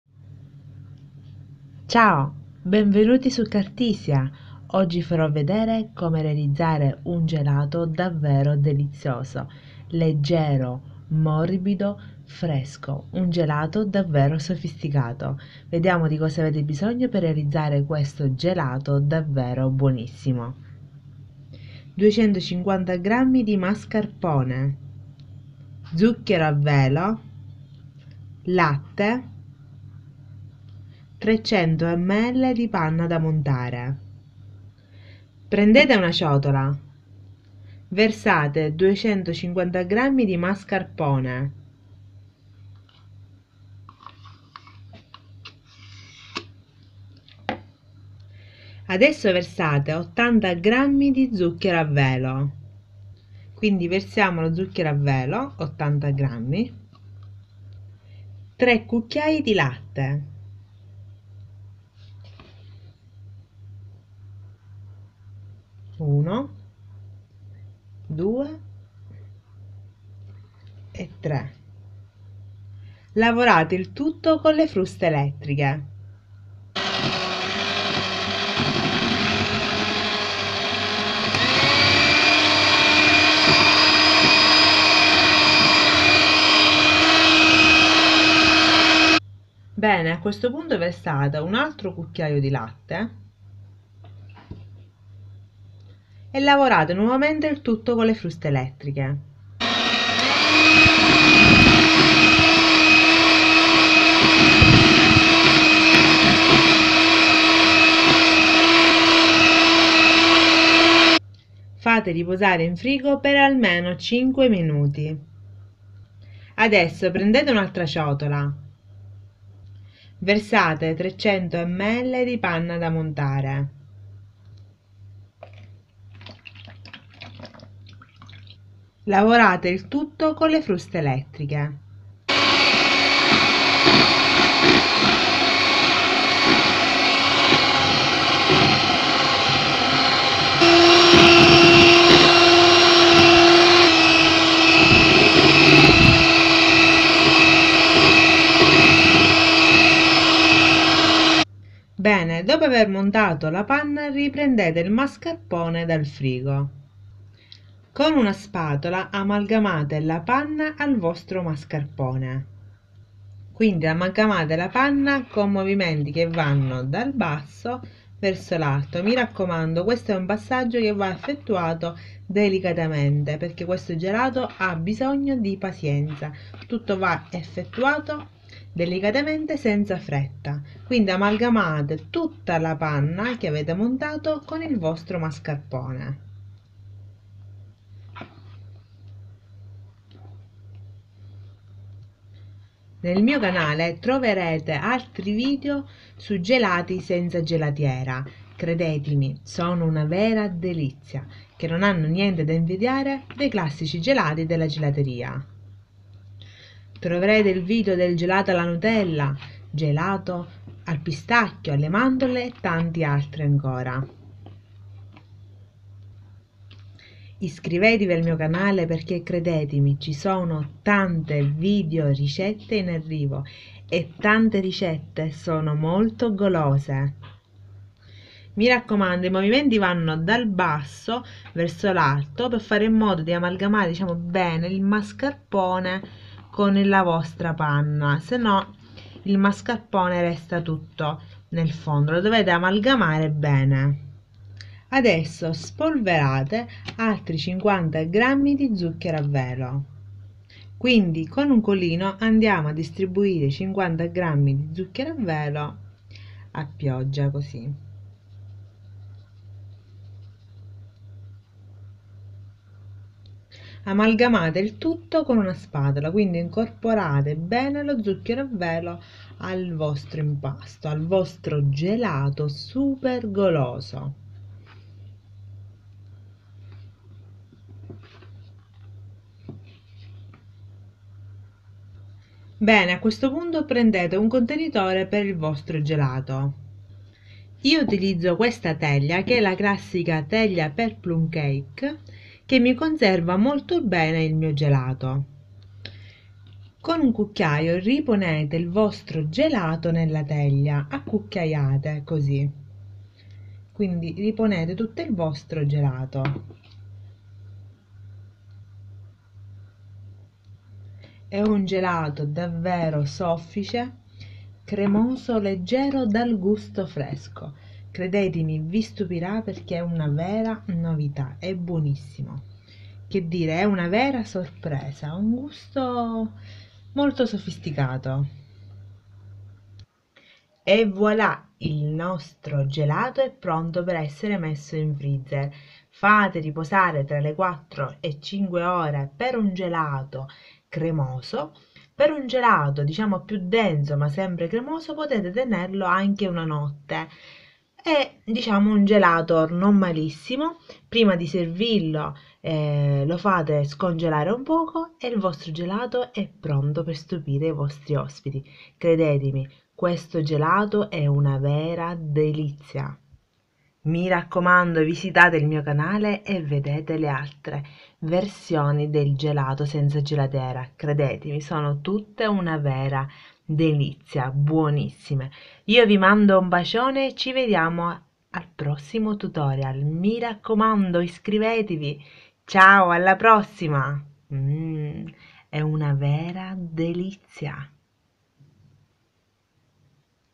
Gelato Nuvola Bianca senza gelatiera
Avete mai provato a preparare in casa il gelato senza gelatiera?
Oggi vi propongo il gelato nuvola bianca,dal gusto soffice morbido buonissimo... sembra di assaporare una vera nuvola soffice..